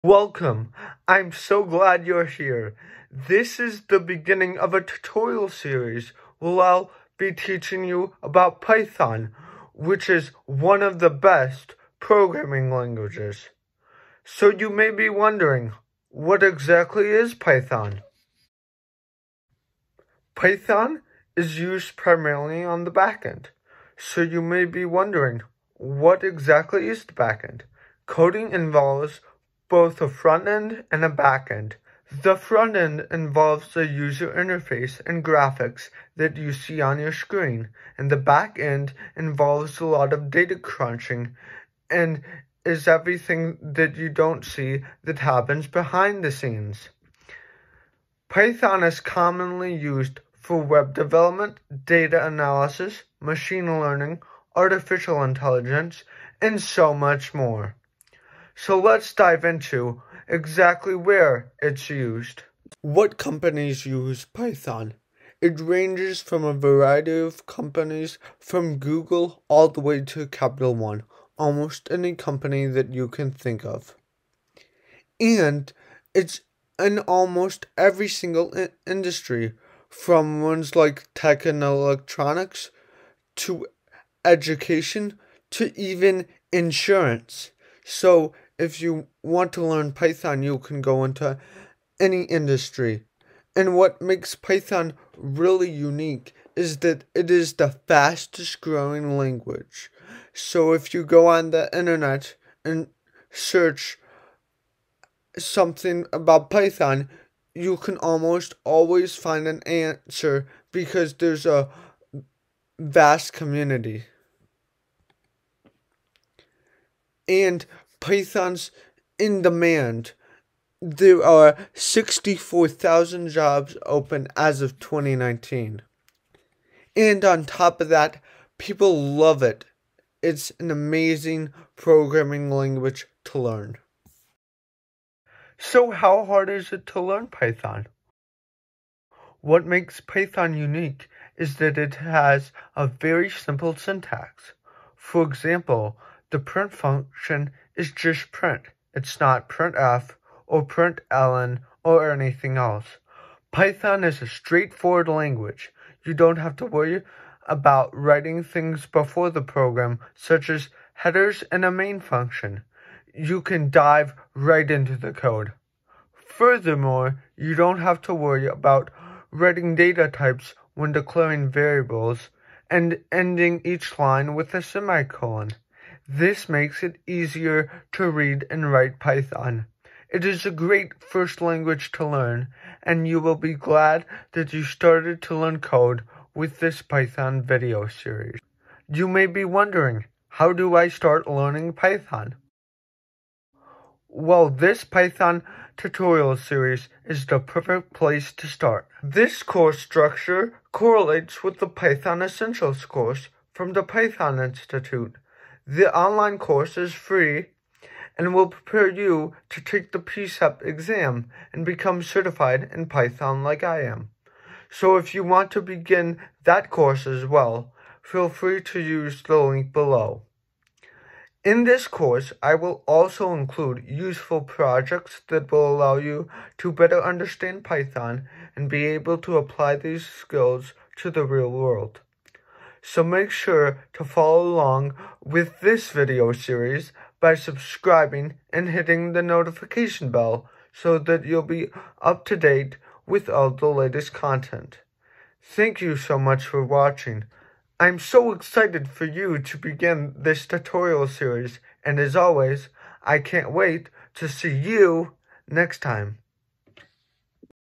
Python Tutorial 1: What is Python?
Do you want to start learning Python? Now is the time! This is the 1st of a series of Python Tutorials. In this series, you will start with learning the basics of Python and eventually be able to write your own complex programs!

Timecodes
0:00 - Welcome
0:18 - What is Python?
2:18 - Python is Easy to Learn
3:12 - How to Start Learning Python